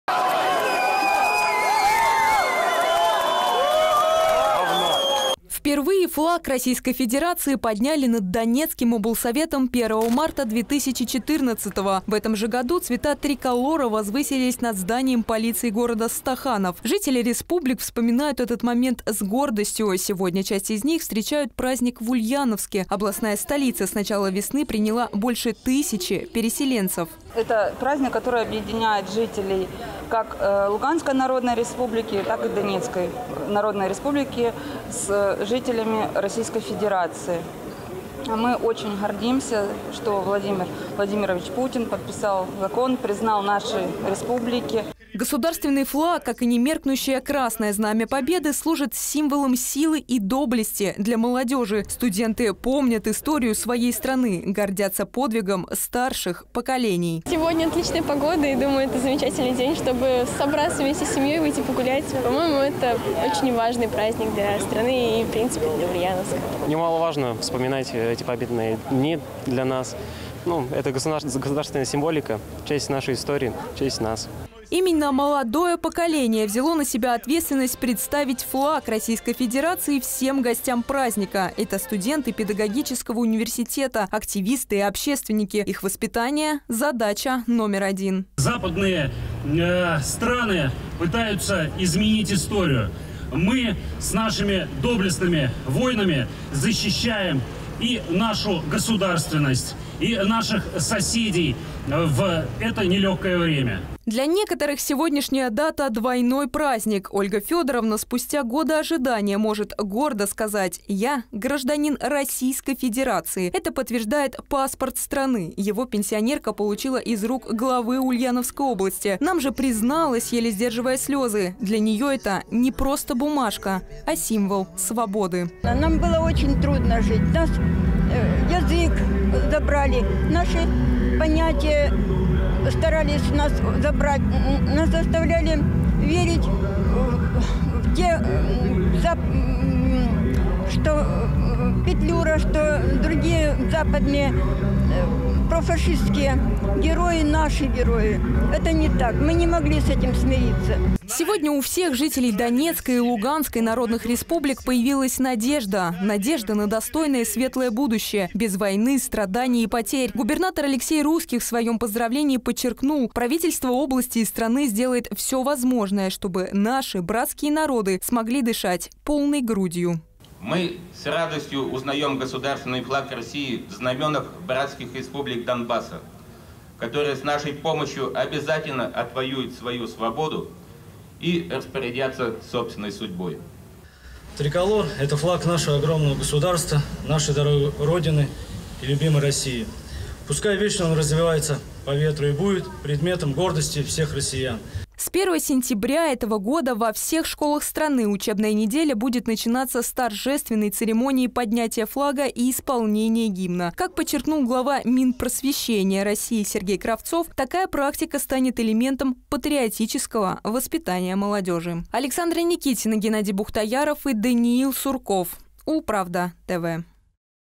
Ожидание, радость, гордость. Жители ЛНР получили паспорта и отметили День флага в Ульяновске
Вся страна окрасилась в цвета триколора. В России отмечают День Государственного флага. Праздник утвердили в 1994 году. В своем поздравлении Владимир Путин говорил о преемственности поколений и о том, насколько это важно для такой большой и мощной державы. В Ульяновске к торжеству присоединились жители из ДНР и ЛНР. Для них сегодняшняя дата – двойное событие.